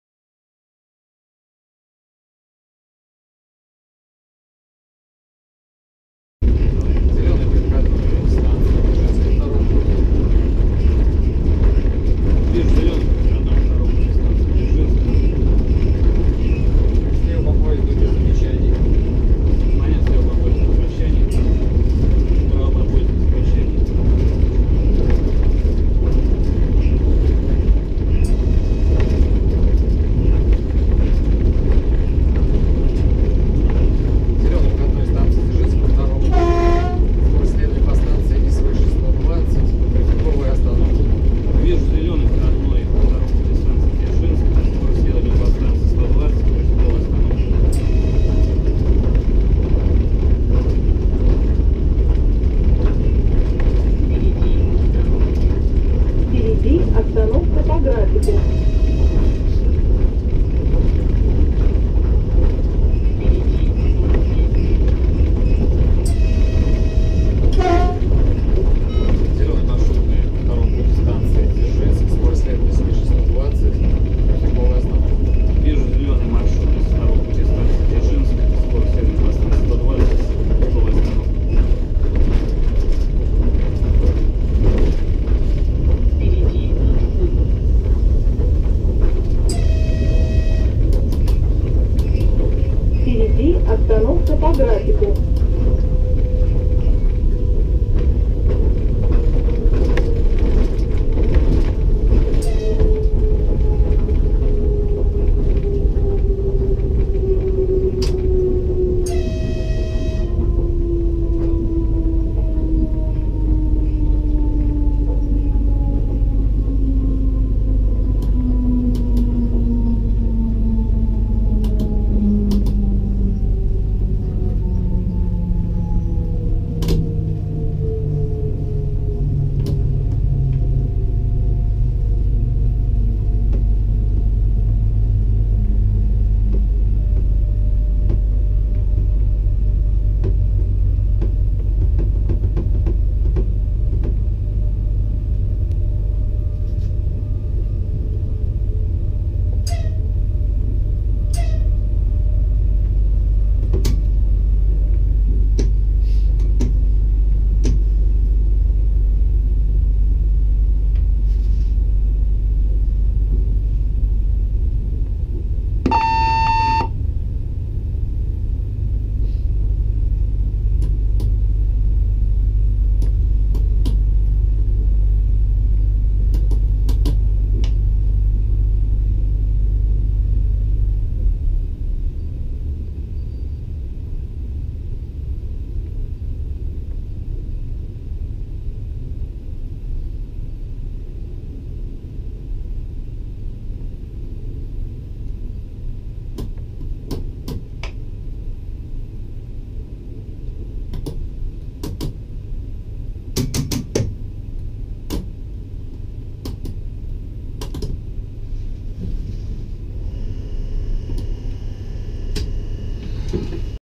Плавный заход на посадку в Дзержинск со 140 км/ч
Заход на посадку в Дзержинск со 140 км/ч с локомотивом ЭП20.